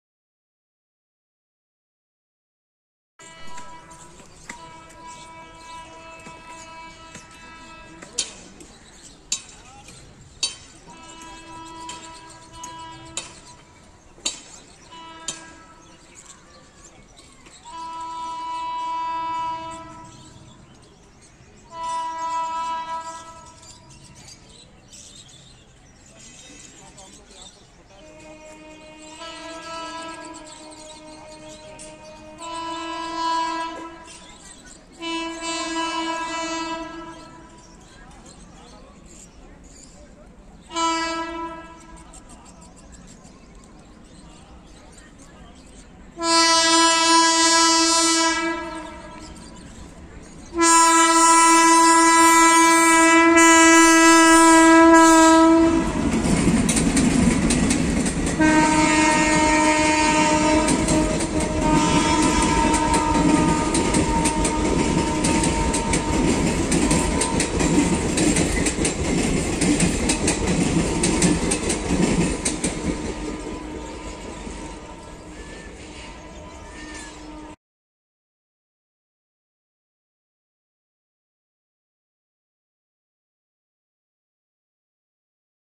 IRFCA | Howrah bound Mayurakshi accelerates after skipping Bally | WAP5
Train : 03046 RPH HWH Mayurakshi Special
Locomotive : Howrah WAP5 30091
Locatoin : Bally
Device : Redmi Note 9 Pro Max